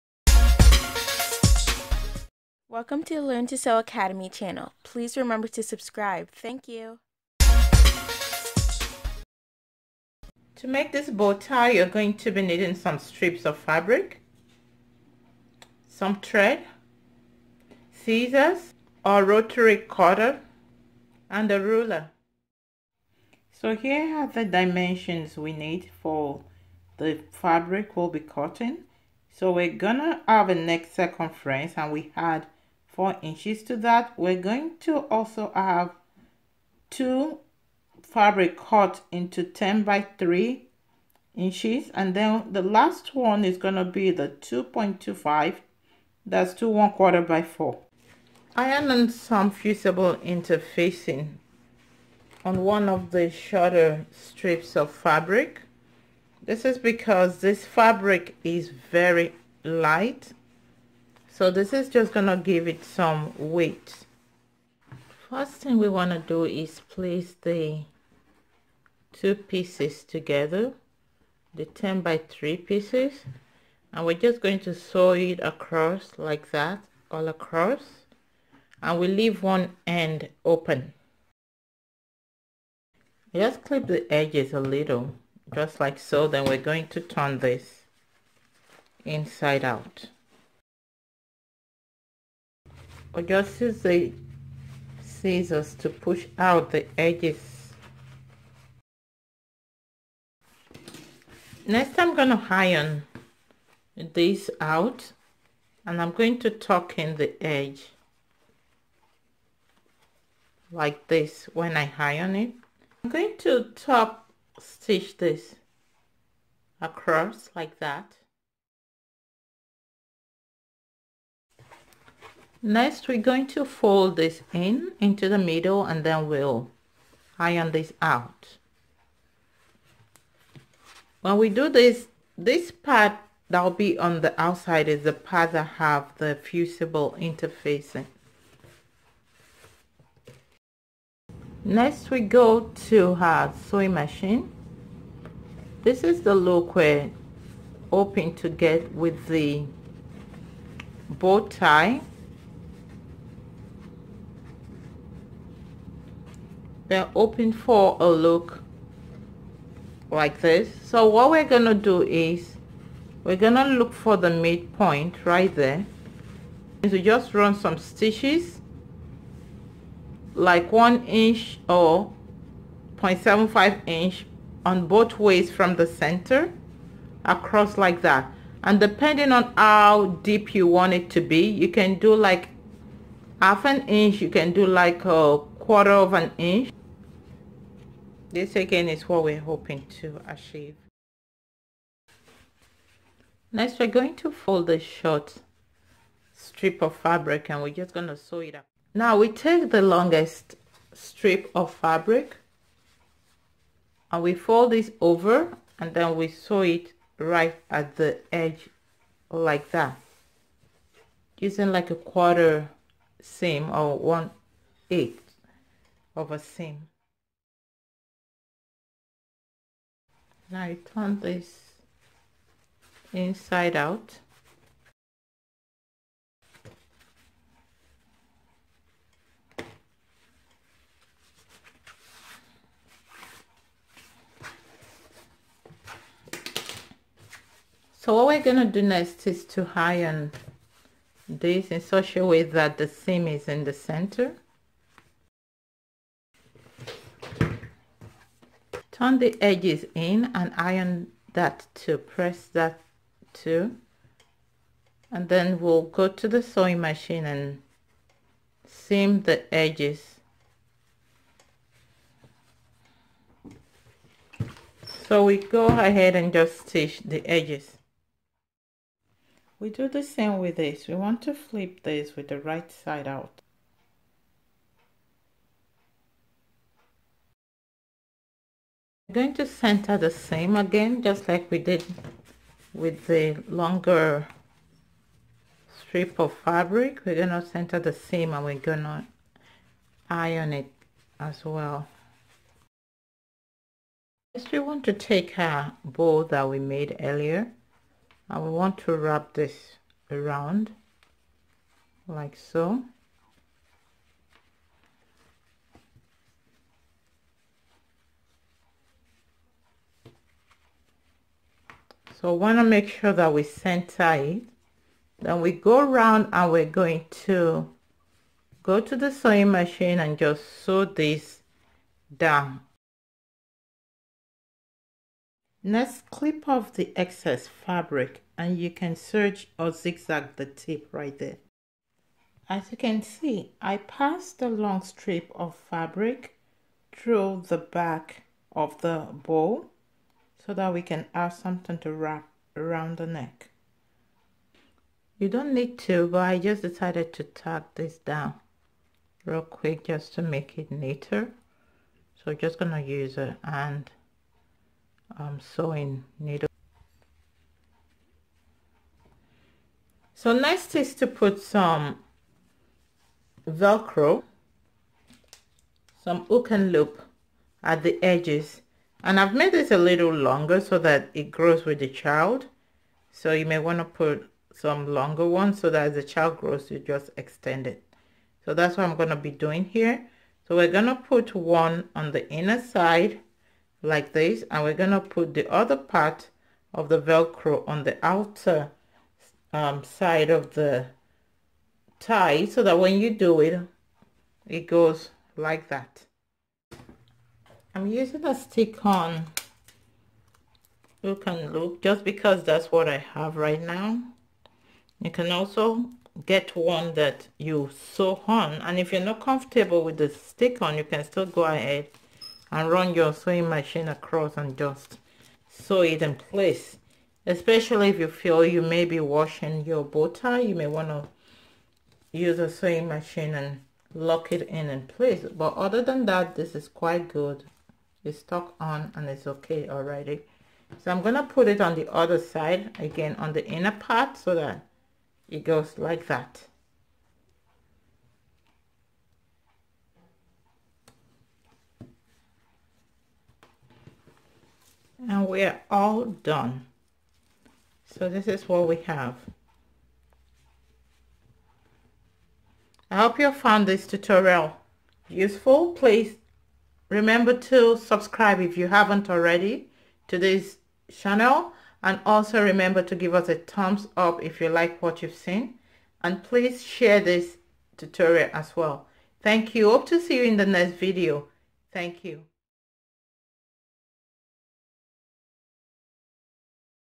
How to Make a Bow Tie | EASY DIY PROJECTS | LTS Academy
How to Make a Bow Tie. In this video, you'll learn how to sew a bow tie for kids; the same process goes for sewing a bow tie for adults.

Sometimes you may need a bow tie but can't find the color you need. This tutorial shows you how easy it is to make one!

Get notifications of new videos by clicking the bell icon. Thank you for your support!  :)

Want your outfits to dazzle and sparkle? Watch our video on adding rhinestones (crystals) to your clothes at: